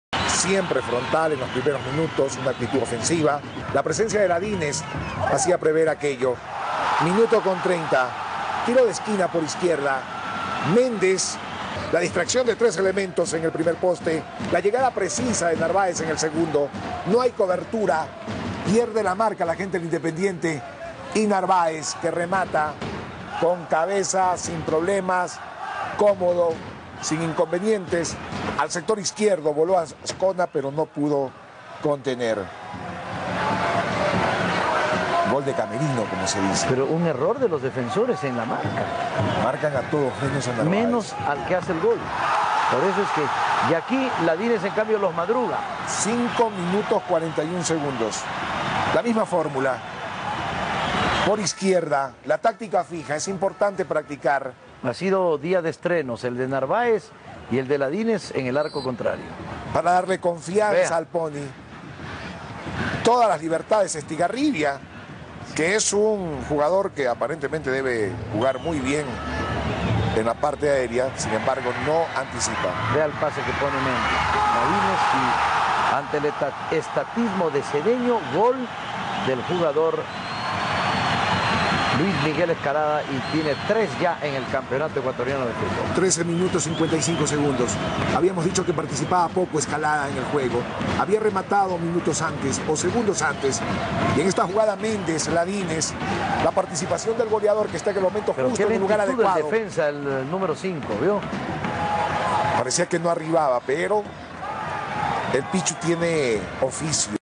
Deportivo Cuenca 3 - Independiente José Terán 0 - Campeonato Ecuatoriano 2010
Con 3 goles antes del primer cuarto de hora, Deportivo Cuenca venció a Independiente del Valle 3x0. El resto del partido fue prácticamente de trámite y, a momentos, estuvo para el bostezo. Con esta victoria, los colorados siguen en el pelotón de punteros con 11 puntos.

Apenas al primer minuto, los locales abrieron el marcador. En un tiro de esquina, Gabriel Méndez levantó el centro el lateral y John Narváez apareció solo como el hombre no programado para cabecear a las redes. 4 minutos después, los colorados aumentaron la ventaja en una acción casi calcada a la del primer tanto. Méndez levantó el tiro de esquina al mismo punto, pero esta vez fue Jorge Ladines quien apareció solo en medio de la zaga para conectar el cabezazo al fondo y bautizarse con la camiseta colorada.

Independiente quiso reaccionar y probó con sendos remates de Armando Wila y Víctor Chinga; el primero se fue apenas desviado y el otro fue atajado con dificultad por Dreer. No obstante la respuesta de la visita, el Cuenca puso la tercera a los 14. Tras un pase de Méndez, Ladines se escapó por la derecha y sacó un centro para que Luis Escalada se barriera y empujara el balón al fondo.

Ya con tres goles, los locales bajaron las revoluciones y no tenían mucha dificultad para contener a su rival. Pese a ello, a los 32, los del Valle estuvieron cerca del descuento cuando, en una jugada en el área, José Ayoví apareció para sacar un disparo que se fue muy cerca del poste.

Para el segundo tiempo, el partido se tornó aburrido. El Independiente no hacía mayor esfuerzo por buscar la hazaña y el conjunto local, ya con la victoria en el bolsillo, tampoco se esforzaba por presionar. Sin embargo, serían los locales quienes estuvieron más ceca de la cuarta que su rival del descuento. A los 64, los cuencanos levantaron en algo a su hinchada cuando Cristian Cordero estrelló un cabezazo en el horizontal tras centro de Bóhorquez. A los 70, el mismo Corcho sacó un disparo que Azcona desvió con una gran reacción.

A los 89, Gabriel Méndez estuvo más que cerca de poner la cuarta y hubiera recibido un premio justo a su gran actuación de la tarde, pero le faltó un poco de suerte. El volante se escapó por la derecha, llegó solo al área y, ante el achique del golero, buscó el primer palo, pero el balón se estrelló en el poste.

Alineaciones

Deportivo Cuenca: Esteban Dreer; John Narváez, Diego Ianiero, Marco Quiñónez y Juan Guerrón; John García, José Granda, Cristian Cordero (Fernando Fajardo) y Gabriel Méndez; Jorge Ladines (Darwin Caicedo) y Luis Miguel Escalada (Marcelo Bohórquez).

Independiente: Librado Azcona; Sergio Cedeño, Reinaldo Román y Yason Corozo; Víctor Chinga, Álvaro Carcelén, Domingo Martínez (Eddy Ramírez), Daniel Samaniego (Paúl Santamaría) y José Ayoví (Johnny Baldeón); Armando Wila y Richard Estigarribia.